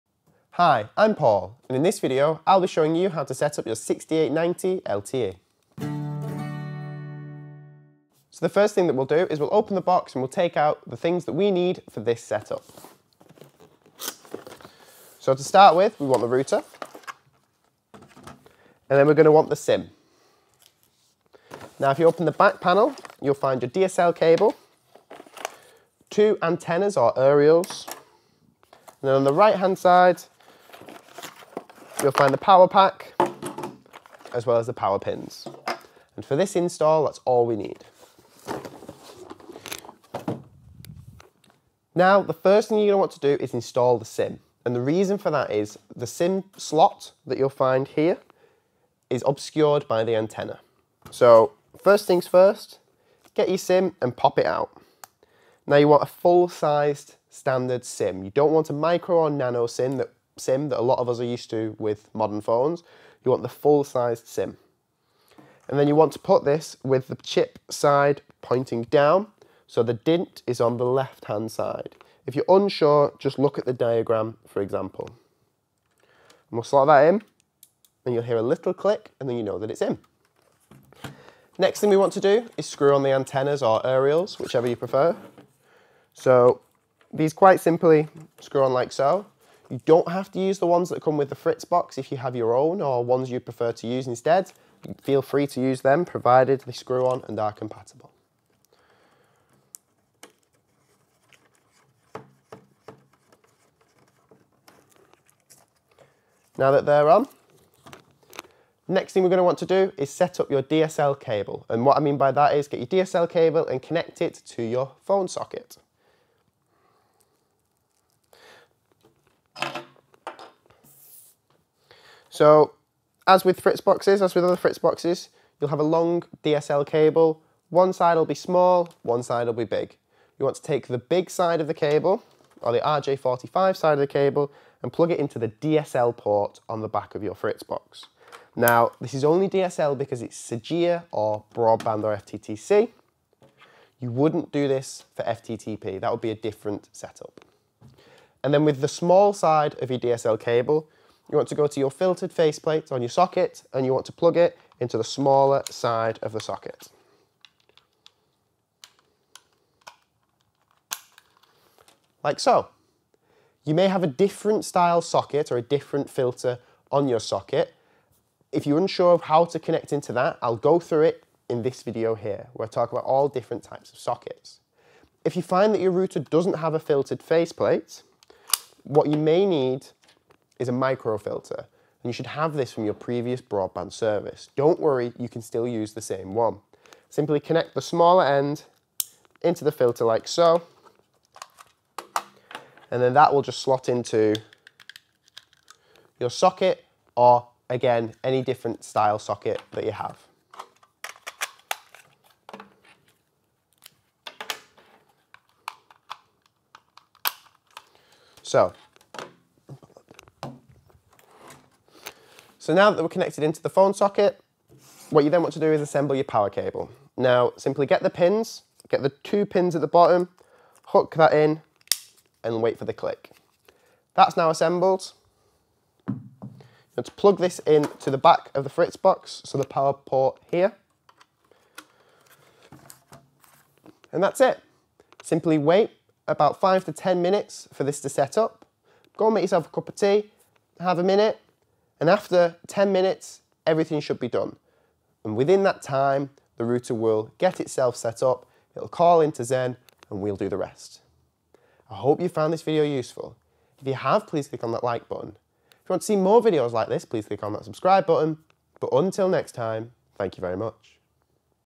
How to Setup your Fritz!box 6890LTE for the First Time for SoGEA
In this video Paul will be showing you how to setup your new Fritz!box 6890LTE for your SoGEA service the first time.

As with all of these check its a good idea to have a pad and paper to hand to make notes of any steps you complete should you need to refer back to them later.

What you will need:

* Time
* Access to your BT / Openreach Master socket